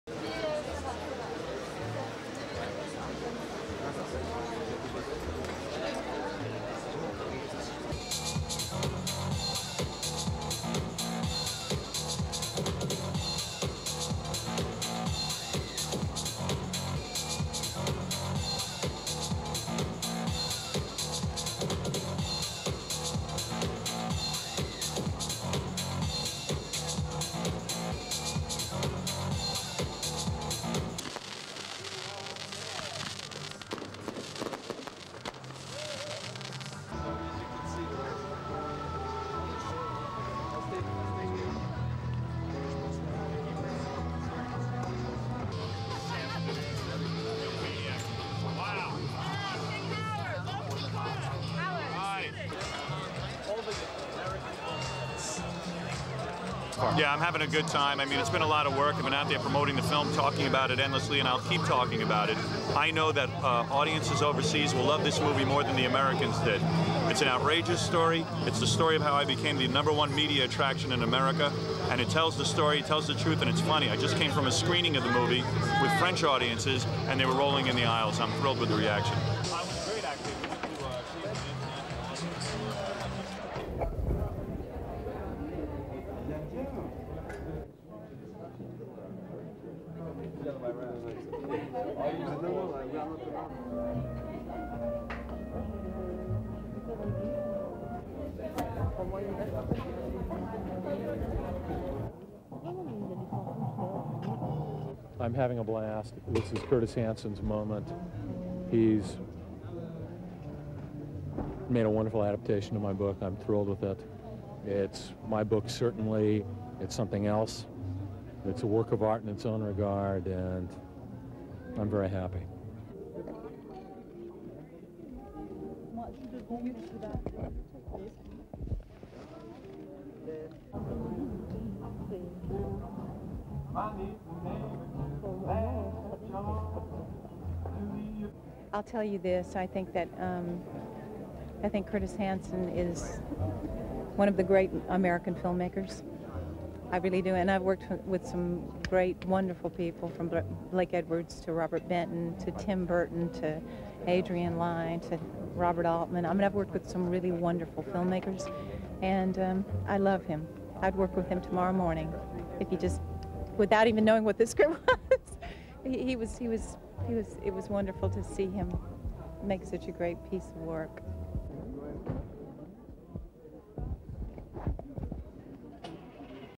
FRANCE: CANNES FILM FESTIVAL: PARTIES
Partying and film-making have become synonymous with the world's glitziest film  festival in Cannes which is celebrating its fiftieth year.

The hot-house atmosphere of Hollywood stars, movie moguls and industry players have  made this annual cinema extravaganza the most celebrated event of its kind.

For the thousands who descend on this southern French resort each year - whether  you're in the film industry or not - it's THE place to see and be seen.

The annual Cannes film festival is not just about cinema.

Rubbing shoulders with the right people and being seen in the trendiest places can  prove a useful career boost - and for many the chance of their "big break".

Throughout the festival, when you're not watching films it seems there's only one thing  to do - and that's party!

It's no coincidence Cannes has come to be known as the glitziest, most fun-filled event  of its kind.

But still, the movie-makers have to take the occasional break from the seemingly  endless round of parties in order to do some work.

The notorious New York "shock jock" Howard Stern is in Cannes promoting the film  "Private Parts", which is based on his life.

In the film millionaire Stern joins forces with his radio colleagues - who all play  themselves - to provide a fly-on-the-wall account of their daily antics in the studio.

Stern's controversial radio show has been a feature of the New York airwaves for several  years, characterised by his outrageous interviews and abuse of listeners.

SOUNDBITE: (English)
"Yeah I'm having a good time, its been a lot of work I'm out here promoting the film  talking about it endlessly and I'll keep talking about it. I know that audiences overseas  will love this movie more than the Americans did. It's an outrageous story, it's the story  of how I became the number one media attraction in America and it tells  the story, it  tells the truth and it's funny. I just came from a screening of the movie with French  audiences and they were rolling in the aisles. I'm thrilled with the reaction."
SUPER CAPTION:  Howard Stern, Disc-Jockey

In Cannes, every film has to have a party.  The one thrown for "L-A Confidential", the  new film by director Curtis Hanson, was attended by Hollywood stars like Danny De  Vito and Kim Basinger.

Basinger plays a high-class hooker in the film, based on a novel by James Ellroy.

SOUNDBITE: (English)
I'm having a blast. This is Curtis Hanson's moment, he's made a wonderful adaptation of  my book, I'm thrilled with it. It's my book certainly but it's something else. It's a work of  art in its own regard. I'm very happy"
SUPER CAPTION: James Ellroy, author of LA Confidential

And Kim Basinger was full or praise for the film's director too. .

SOUNDBITE: (English)
"I'll tell you this, I think that Curtis Hanson is one of the great American film makers, I  really do. I've worked with some great wonderful people from Blake Edwards to Robert  Denton to Tim Burton to Adrian Line to Robert Altman, I mean I've worked with some  really wonderful film makers. I love him, I'd work with him tomorrow morning, without  even knowing what the script was. It was wonderful to see him make such a great piece  of work."
SUPER CAPTION: Kim Basinger, Star of LA Confidential

The film, a darkly compelling vision of violence and corruption in fifties America, also  stars De Vito as a tabloid journalist who will stop at nothing to get his story.

In the meantime the real De Vito just joined in the fun - along with all the other  thousands who party the night away in Cannes.